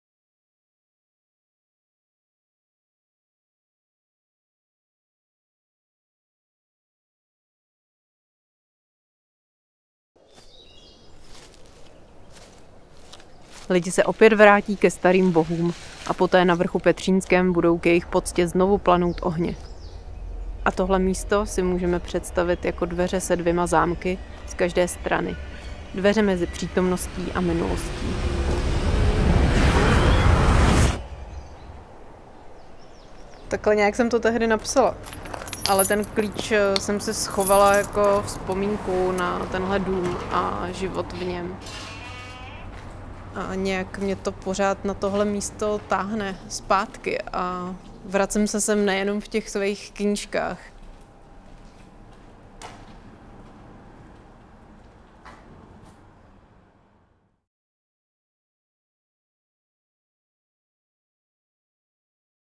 Sweet Home Malá Strana| Malostranský dekameron| Vystěhovaná
Jeden dům na Pohořelci. Osud několika rodin. A jedna konkrétní Vystěhovaná, co si pořád s sebou nosí klíč od svého dětství na Petříně. Dům je stále prázdný. Z velkých plánů se zatím nestalo nic. Ona se na Malou Stranou stále vrací, ať už reálně, nebo ve svých knihách.
„Ten klíč jsem si schovala  jako vzpomínku na tenhle dům a život v něm. A vracím se sem nejenom v těch svejch knížkách,“ říká ona Vystěhována Tereza Janišová.

Poznejte s námi zákoutí Malé Strany. Rubínovský malostranský dekameron vznikl díky rozhovorům a toulkám autorek inscenace Sweet Home Malá Strana Magdalény Rejžkové a Olívie Fantúrové.

A nezapomeňte dorazit na inscenaci Sweet Home Malá Strana! Více na